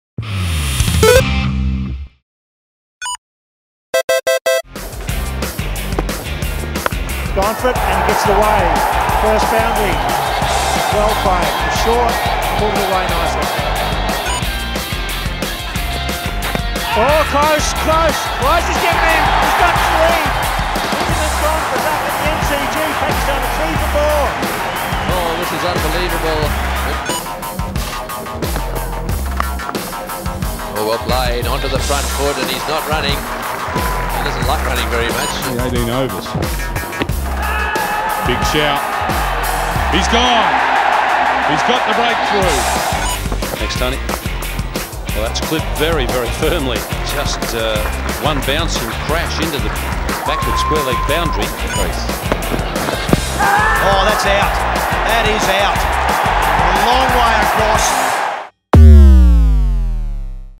Rivalry Week: Glenn McGrath v Inzamam-ul-Haq
Check out the highlights of the rivalry between Inzamam-ul-Haq and Aussie paceman Glenn McGrath, who removed the Pakistan batsman 10 times